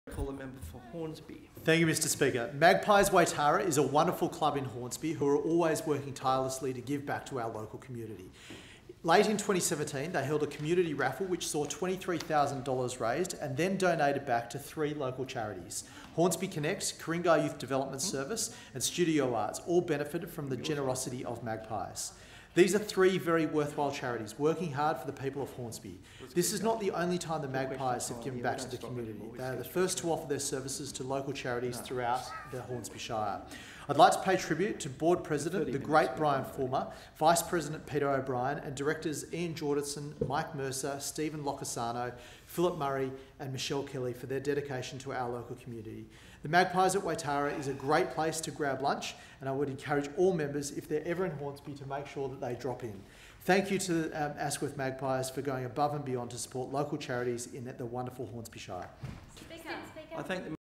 Tribute to Magpies Waitara
Mr MATT KEAN (Hornsby—Minister for Innovation and Better Regulation) (13:44): Magpies Waitara is a wonderful club in Hornsby. Its members are always working tirelessly to give back to our local community. In late 2017 they held a community raffle that raised $23,000 to donate to three local charities. Hornsby Connect, Ku-ring-gai Youth Development Service and Studio ARTES are worthwhile charities working hard for the people of Hornsby and they have all benefited from the generosity of the Magpies. That was not the only time the Magpies have given back to the community; they are the first to offer their services to local charities in Hornsby. I pay tribute to board President Brian Fulmer, Vice-President Peter O'Brien and directors Ian Jordison, Mike Mercer, Stephen Loccisano, Philip Murray and Michelle Kelly for their dedication to our community. Magpies Waitara is a great place to grab lunch; I encourage all members to drop in if they are ever in Hornsby. I thank the Magpies for going above and beyond to support local charities in the wonderful Hornsby shire.